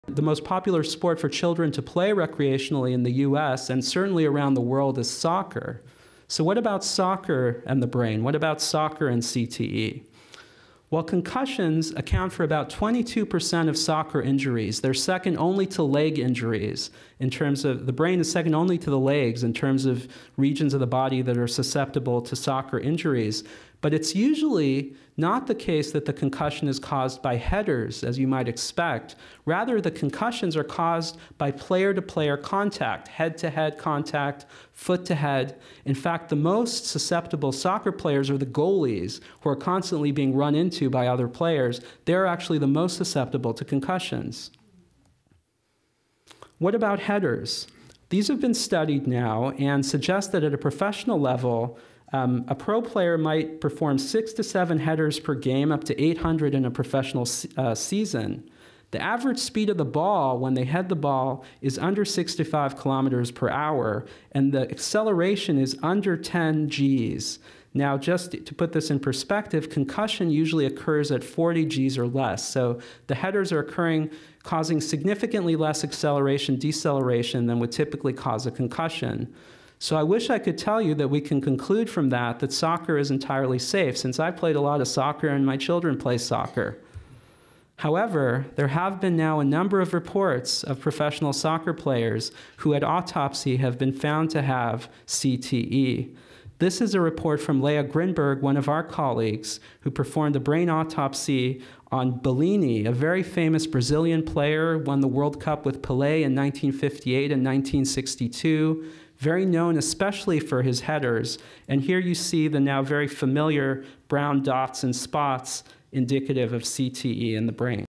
Soccer, the Brain and CTE
Dr. Gil Rabinovici explores CTE, a progressive disorder with symptoms appearing years to decades after exposure to repetitive mild traumatic brain injury (concussions and sub-concussive). Rabinovici discusses CTE's occurrence in the world's most popular sport, soccer. [10/2017] [Show ID: 33036]